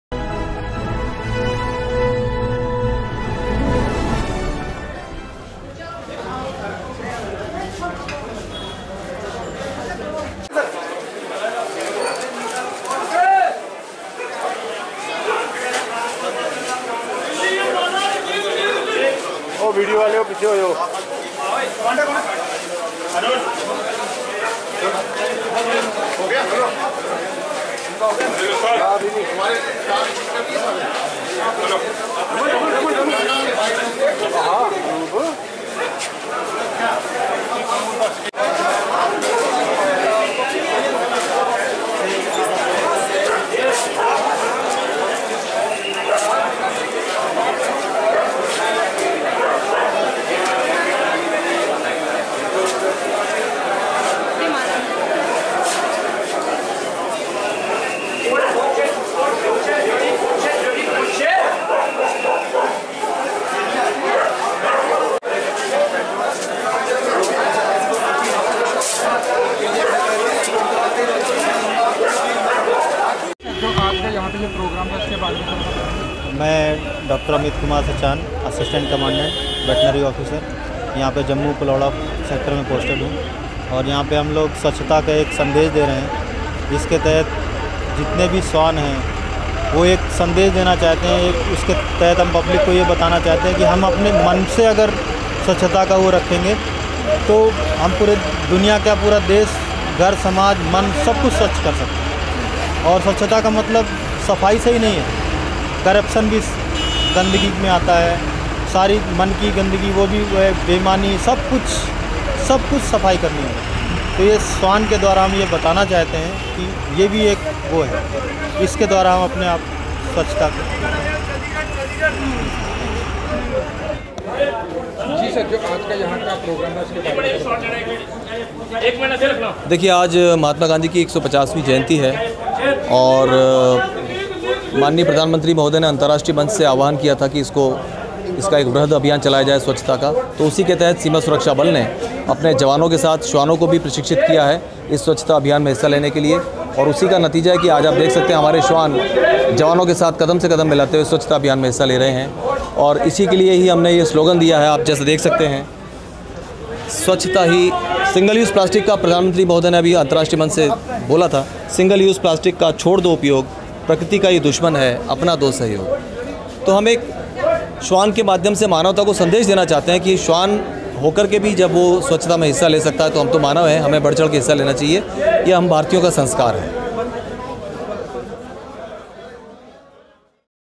JK First News, Today Jammu & Kashmir: Dog squad of Border Security Force (BSF) takes part in 'Swachh
Today Jammu & Kashmir: Dog squad of Border Security Force (BSF) takes part in 'Swachhta Abhiyaan' (cleanliness drive) On Gandhi Jayanti At Bus Stand Jammu.